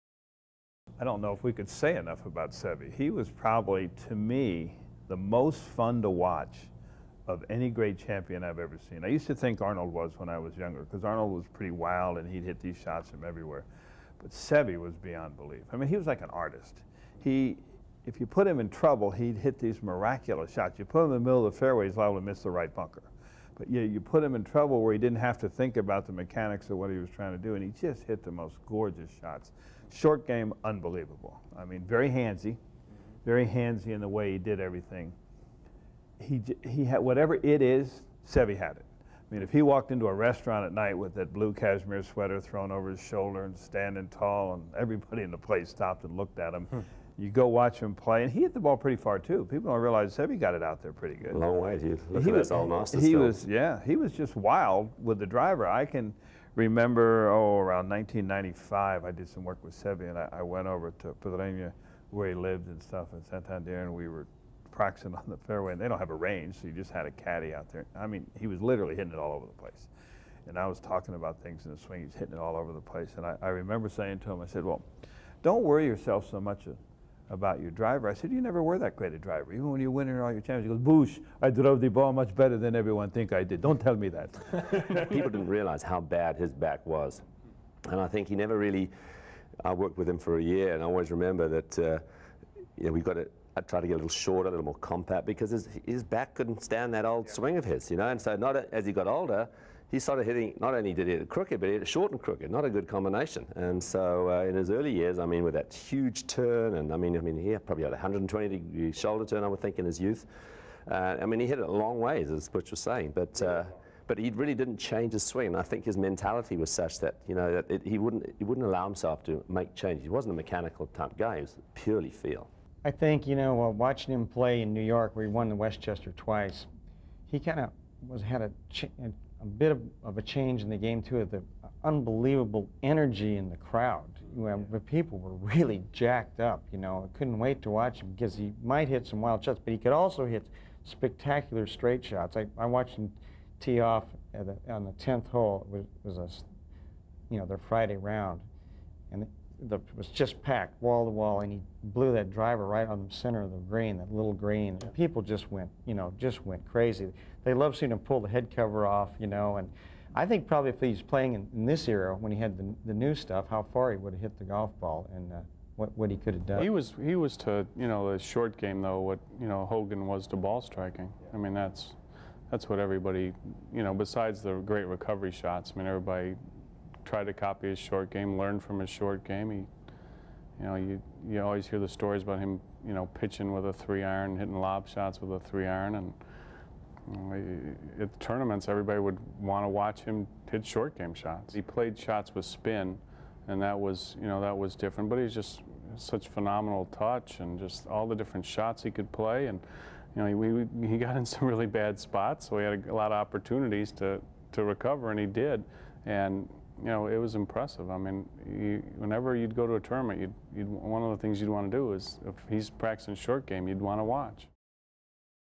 The Big 4: Remembering Seve Ballesteros
From his short game to his dramatic style of play, Golf Digest's top teachers -- Butch Harmon, David Leadbetter, Hank Haney and Jim McLean -- discuss what made Seve Ballesteros such an incredible player.